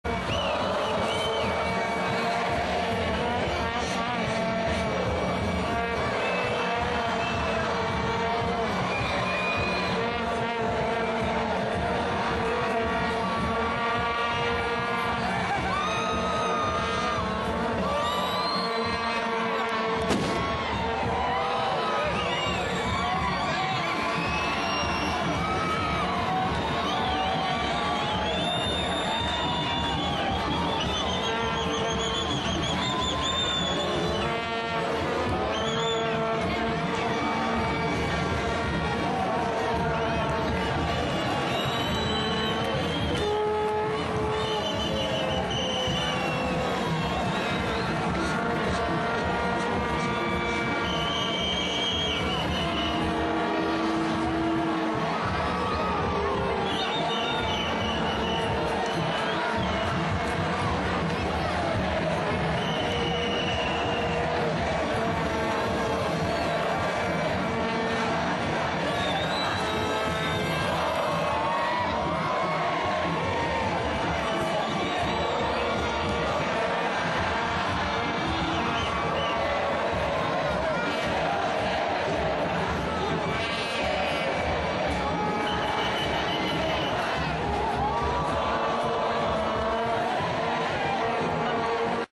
Festejo en Avenida Sarandí, Rivera, del triunfo de la Selección Uruguaya sobre Corea del Sur
octavos de final, copa del mundo SudÁfrica 2010. 26 de junio